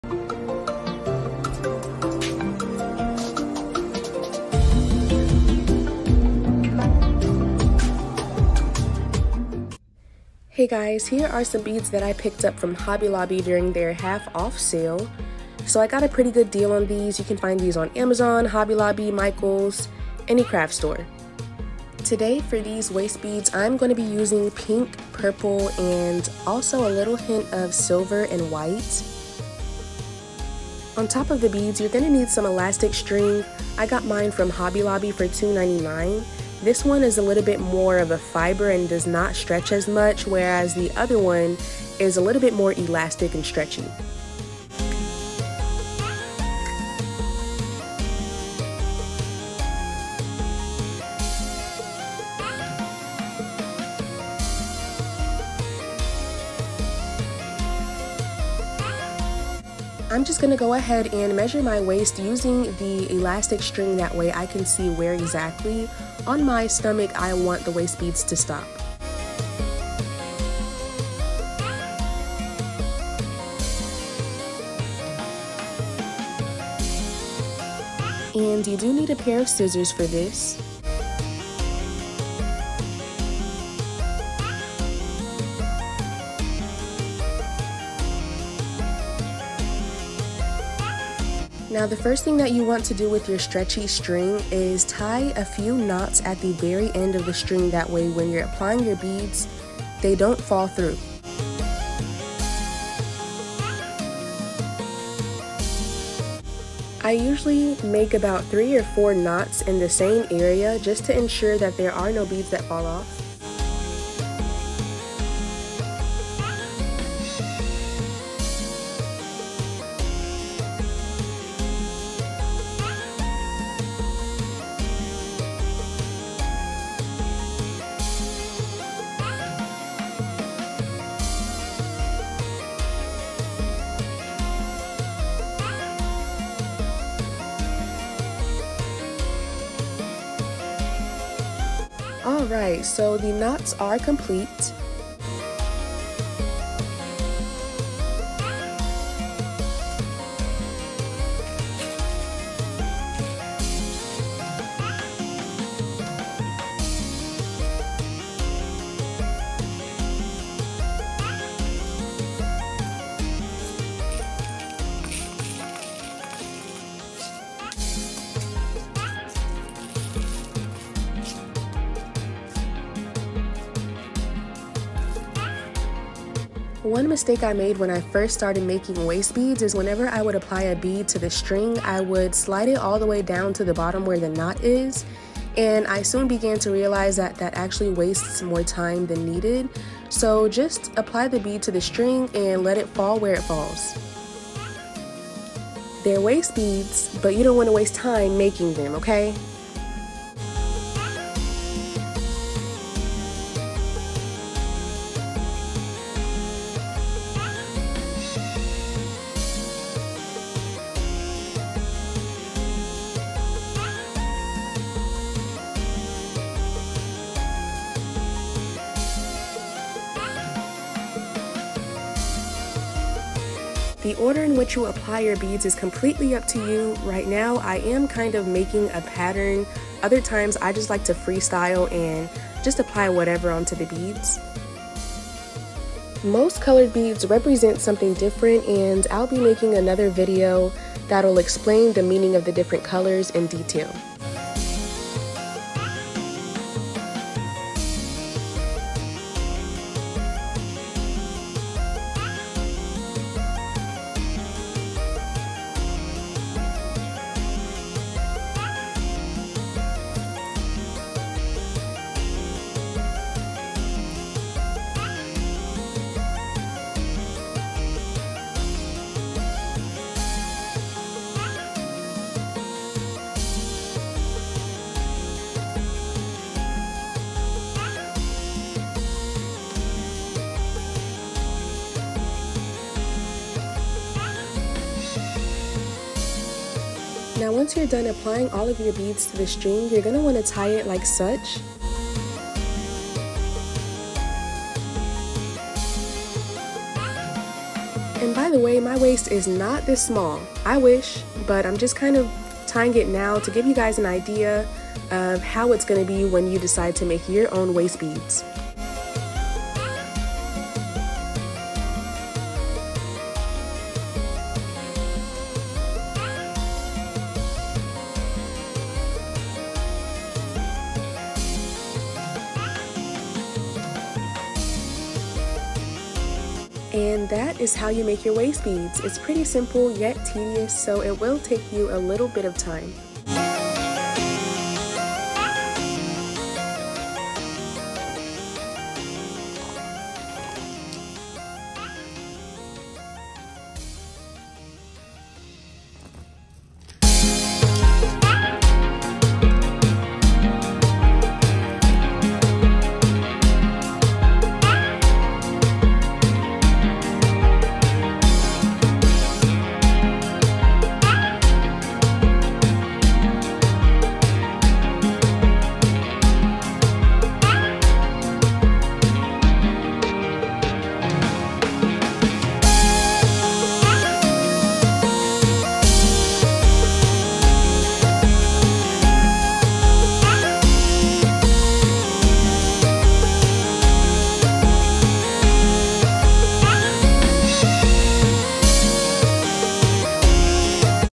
DIY African Waistbeads | How to Tie Waistbeads Tutorial
African Waistbeads have been around since the 15th century. I will walk you through how to make your own meaningful and trendy beads ( :